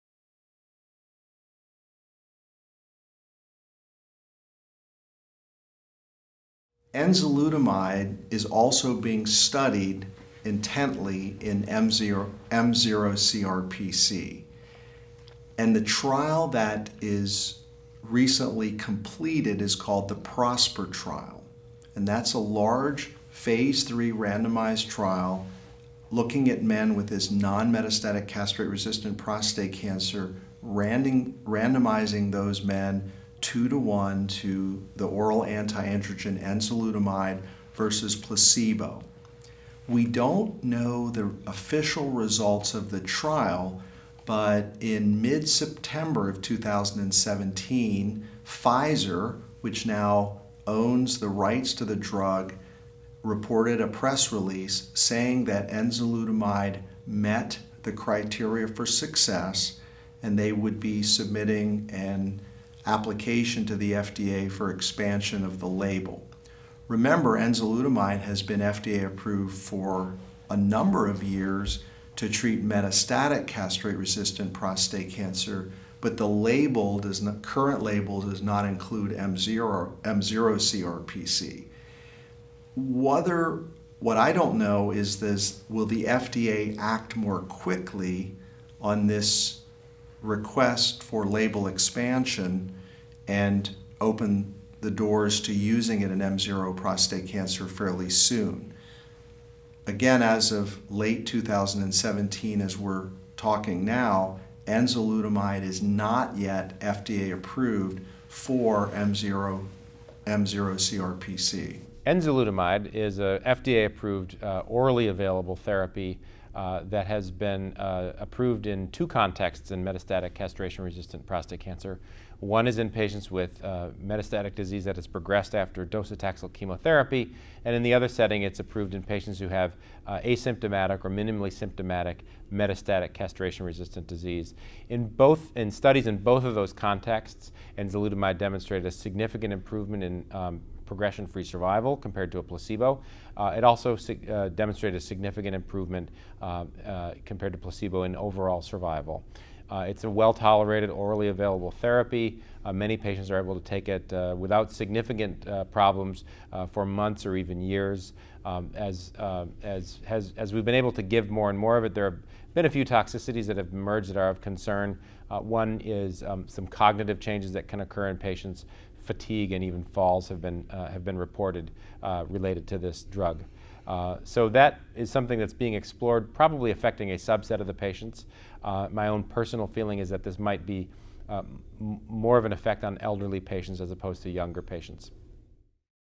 Enzalutamide in the Context of Nonmetastatic CRPC
Judd W. Moul, MD, and Charles J. Ryan, MD, discuss the evolving role of enzalutamide in the context of nonmetastatic castration-resistant prostate cancer.